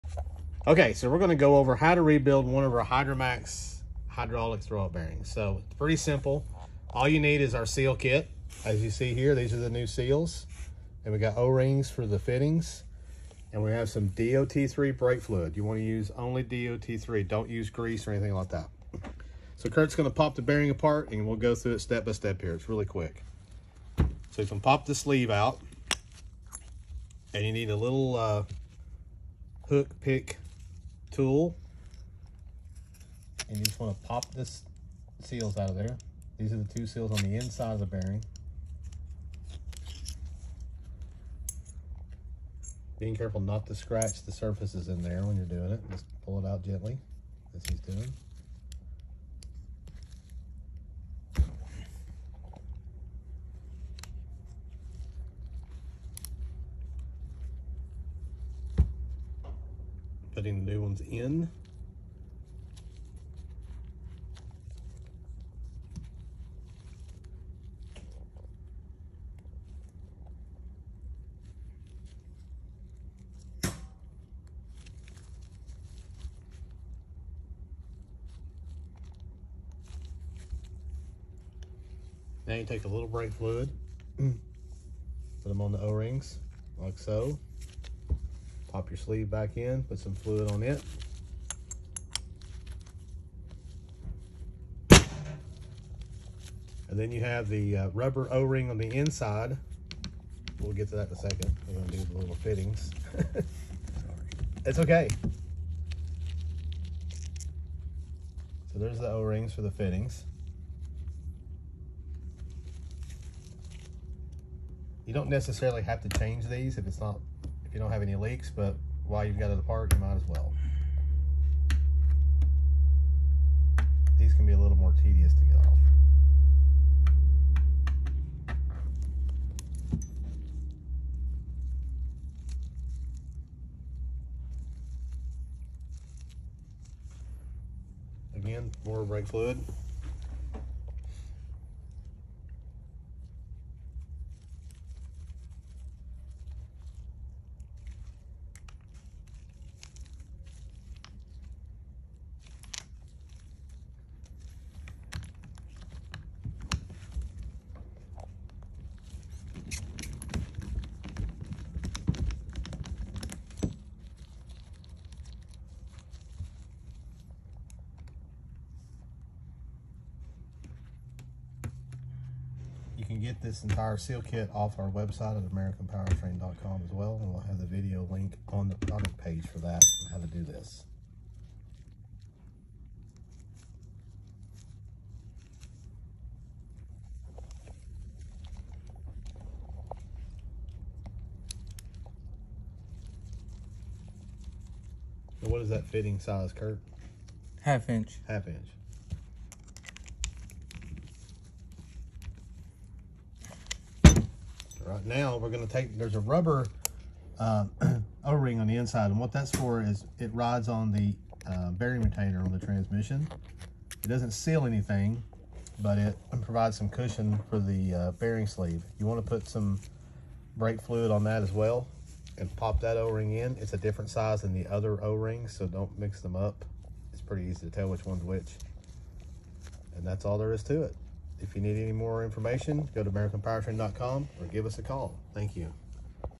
How To Rebuild Your American Powertrain Hydramax Hydraulic Throwout Bearing.
Step by step instructions for rebuilding your American Powertrain Hydramax® Hydraulic Clutch Bearing